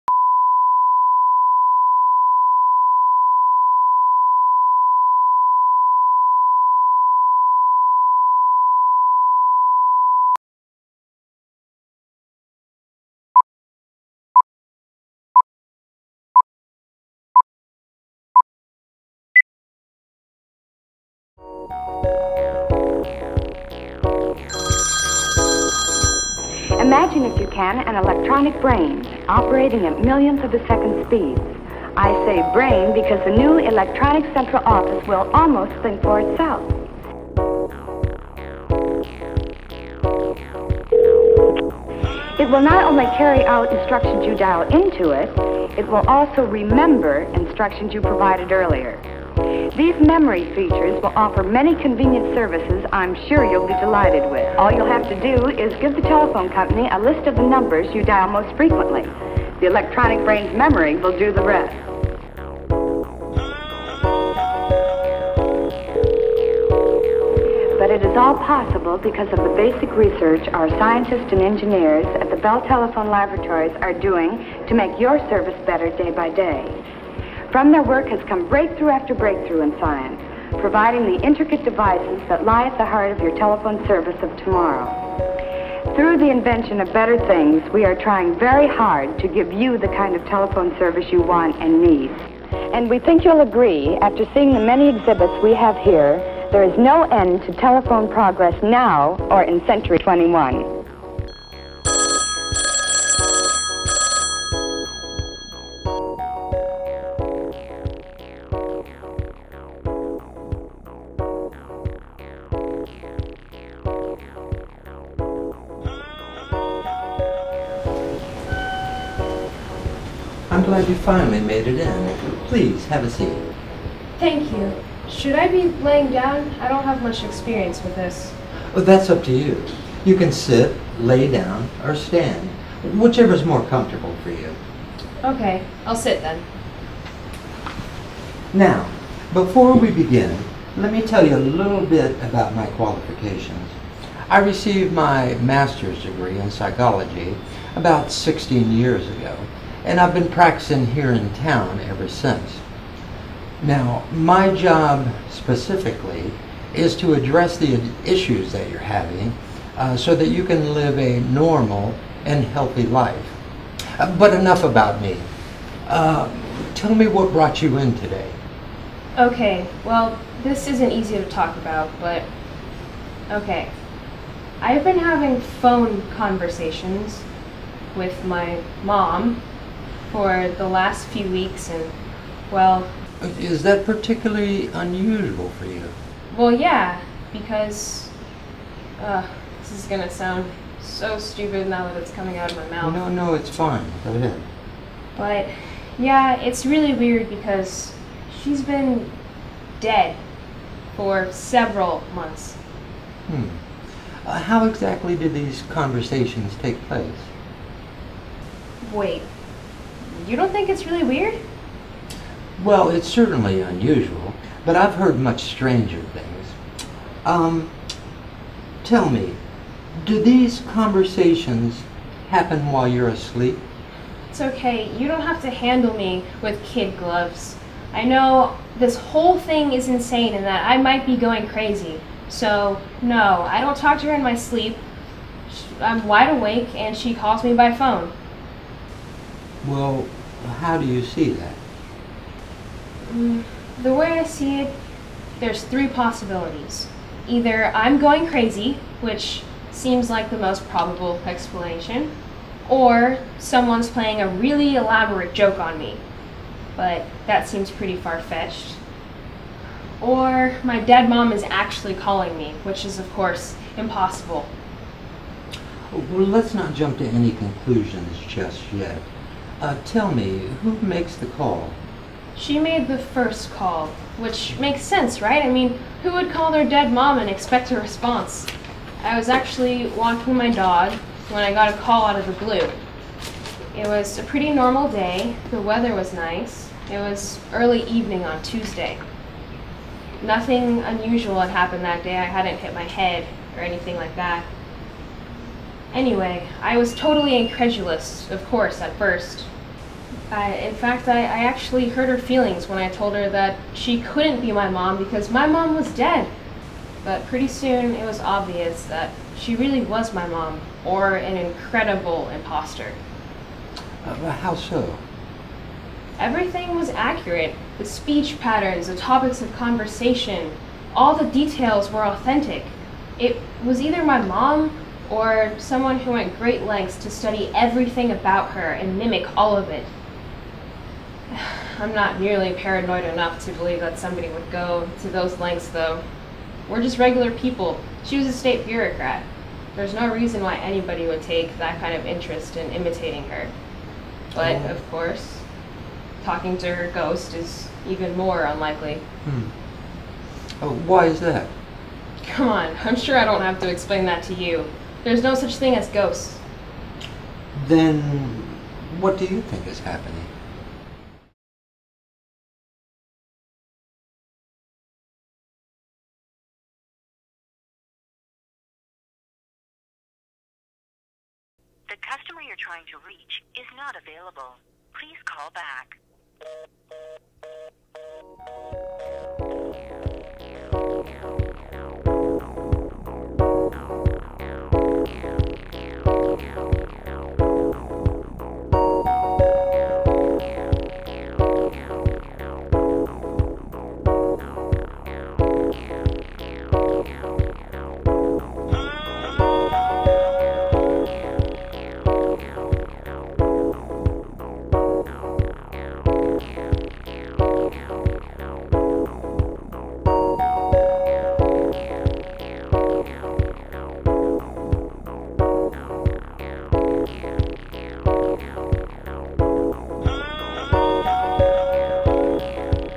Comfort Phone by Matt Goode for ART Gala Preview Full Cut
Comfort Phone by Matt Goode
Directed by Sunny Hughes-Trinity

Auditions: January 17 & 18

Join us for two  locally-written one-act plays in one night.

In Comfort Phone we look at sanity and the supernatural as someone who recently lost their mother receives calls from her and explores that subject with their therapist.Adult themes.

More information and tickets at Acrosstown.org